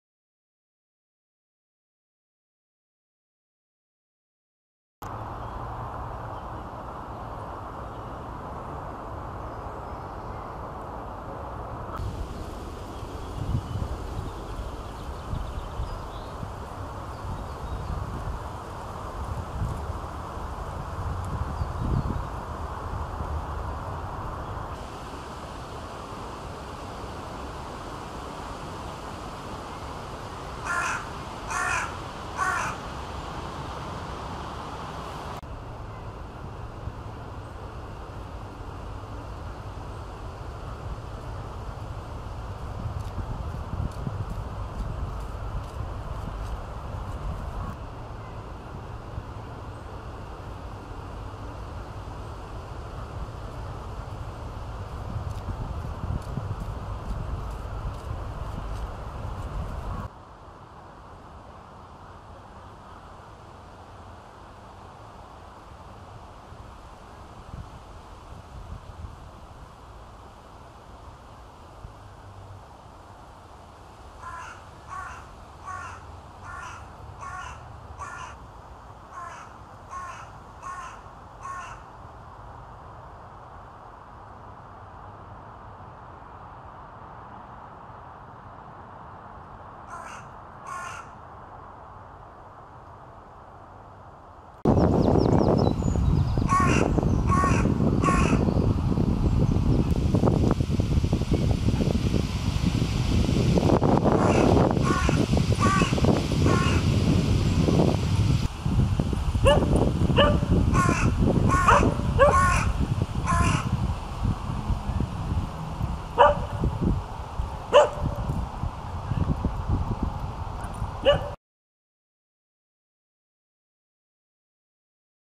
5 Juni 2024 Bonte Kraai langdurig roepend, poetsend in Eik en Populier
De Bonte Kraai van het Scheveningse Bos. Hij liet zich mooi roepend, poetsend en krabbend vastleggen in een Eik en wat later in een Populier op de Zandvlakte..